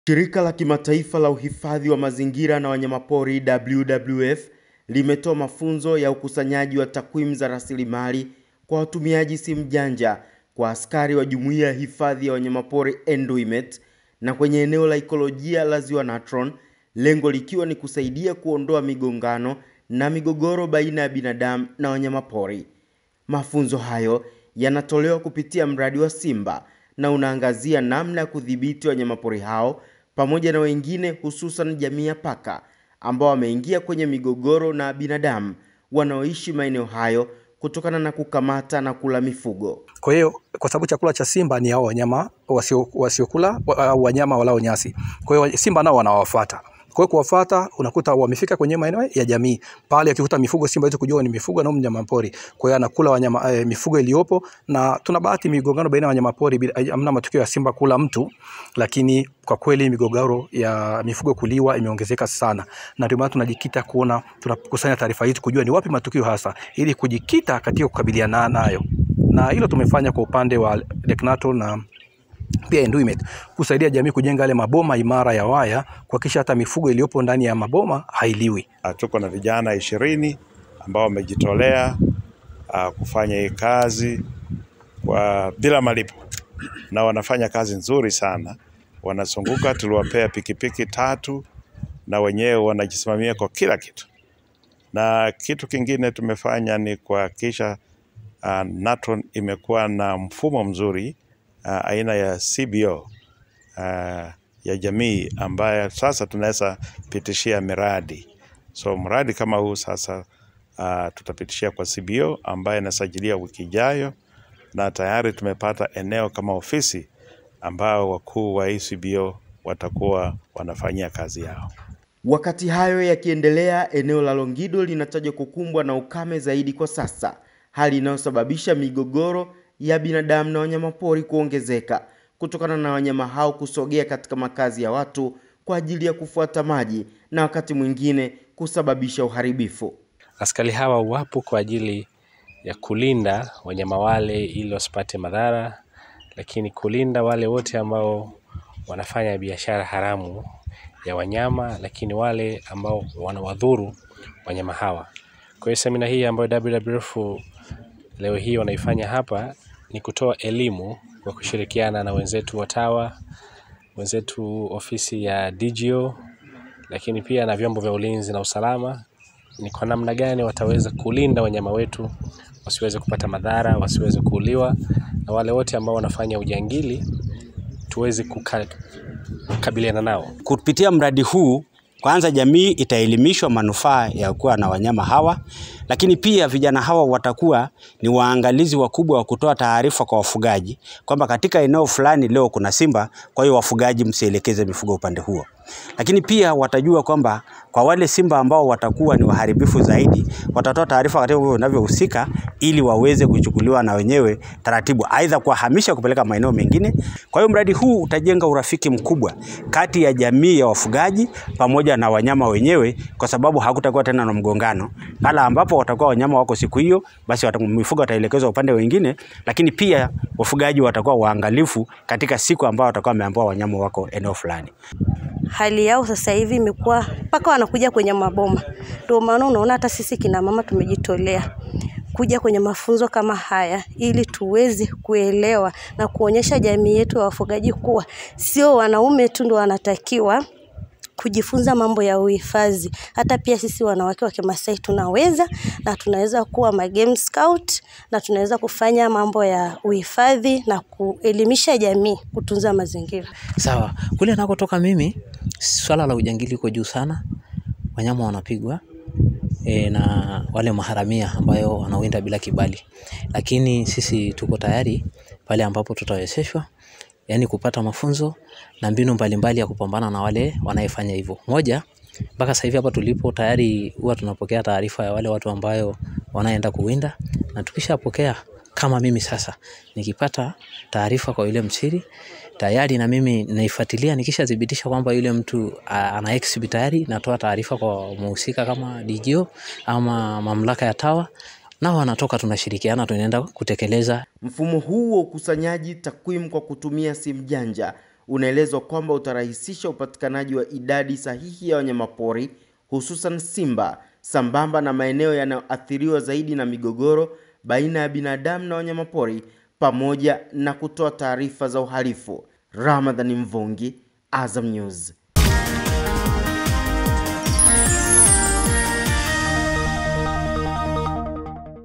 WWF yatoa mafunzo kupunguza muingiliano wa binadamu na wanyama hifadhini
Shirika la Kimataifa la Uhifadhi wa Mazingira na Wanyamapori (WWF) limetoa mafunzo ya ukusanyaji wa takwimu za rasilimali kwa  kwa askari wa Jumuiya ya Hifadhi ya Wanyamapori Enduimet na kwenye eneo la Ikolojia la Ziwa Natron lengo likiwa ni kusaidia kuondoa migongano na migogoro kati ya binadamu na wanyamapori.